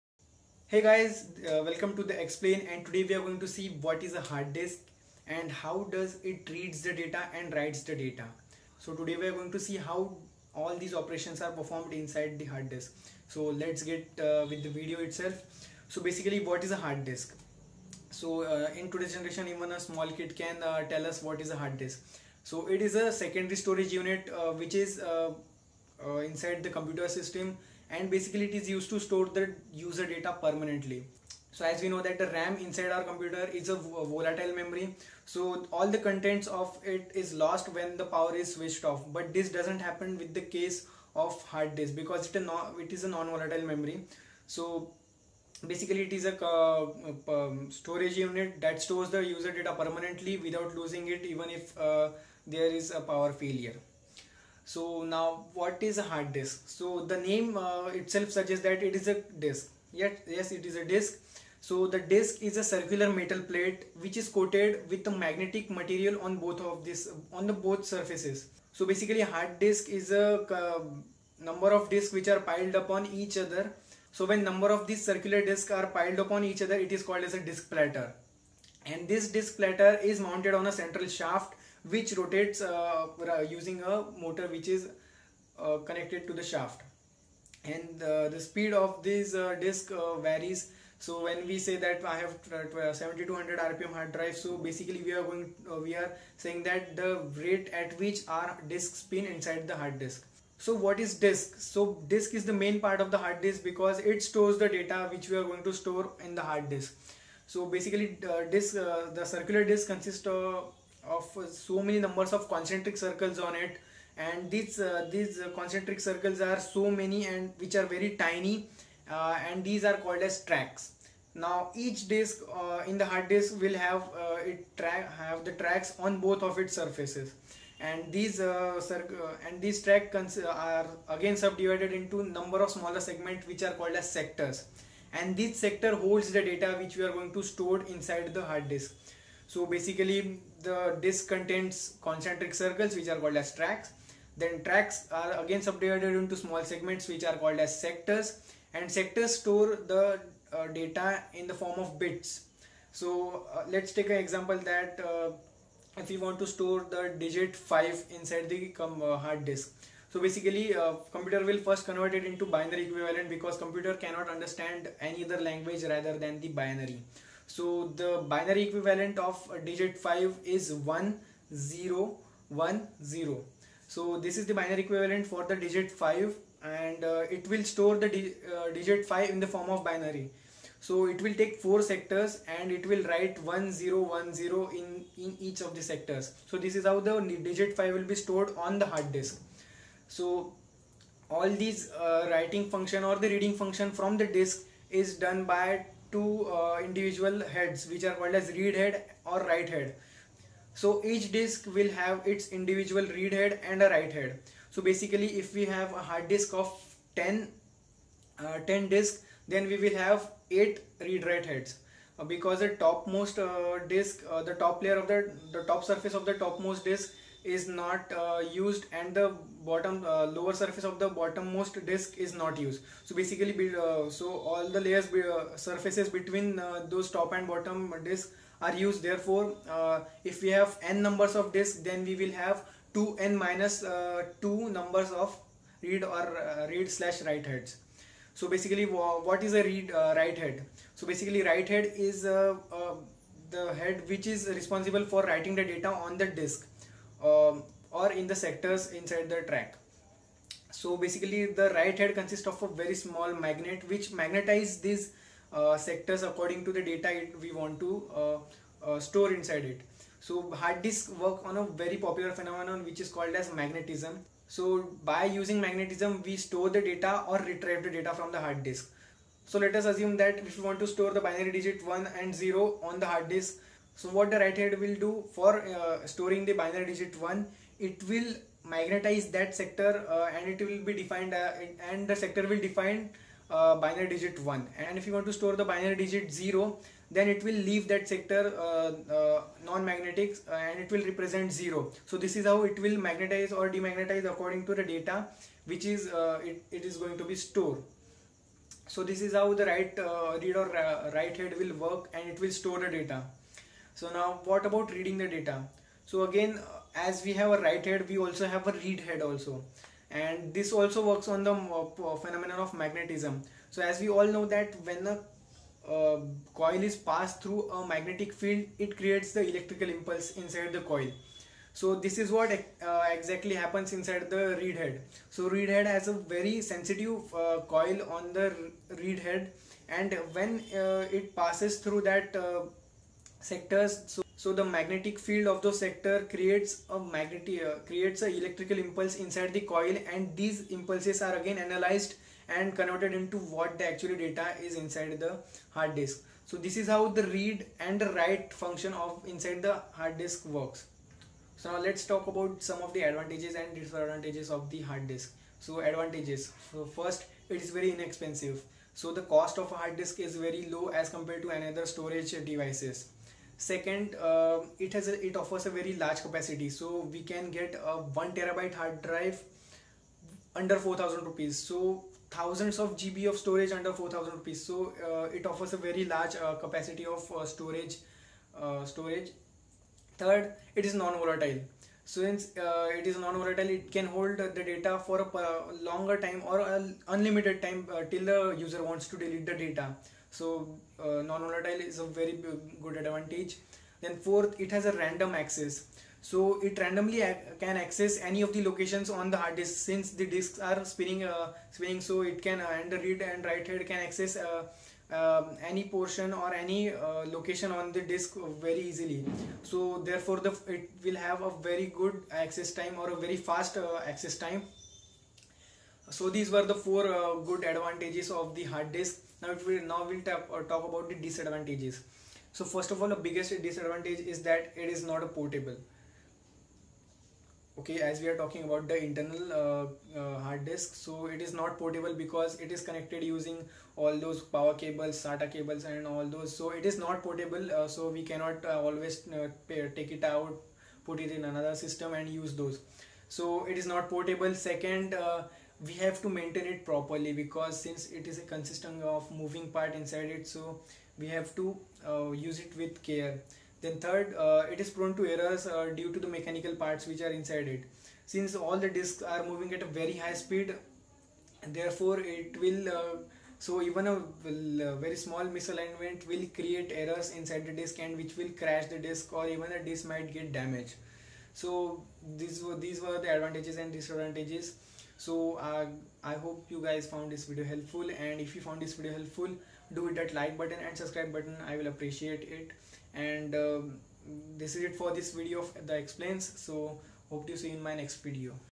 What is Hard Disk? How does it works?
Guys this is new series that i'm going to start i.e The Explain
in this series I'm going to explain various technology related topics with most simplified language and with examples.

Note: Digit 5's binary equivalent is 0101 not 1010.

Today we are going to talk about the HARD DISK's.

Hard Disk's are the most commonly used storage medium in today's world so today let's see how this storage device works

I will also provide some advantages and disadvantages so the topic will more clear.